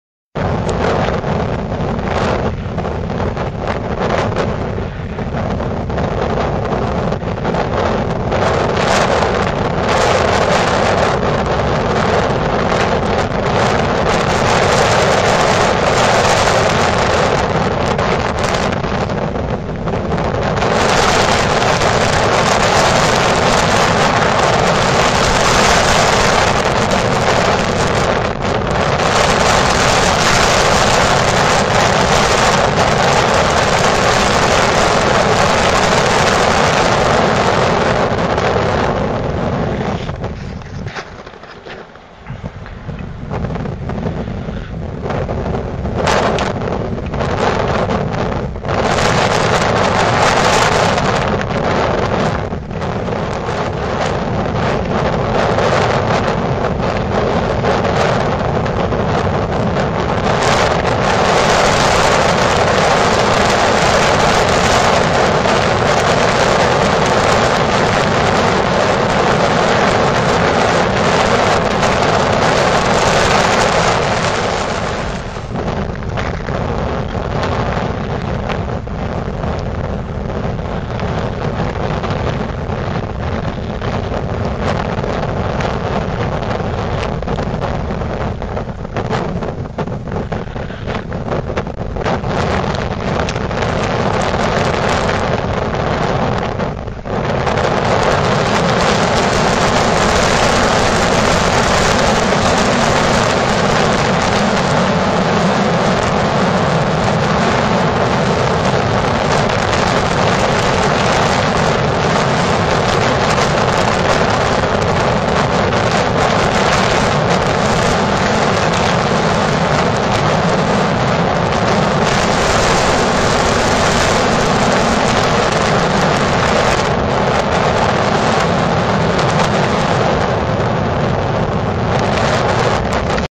Snowshoeing Swains Lake 2
2-17-13 snowshoeing on swains lake. 20 degrees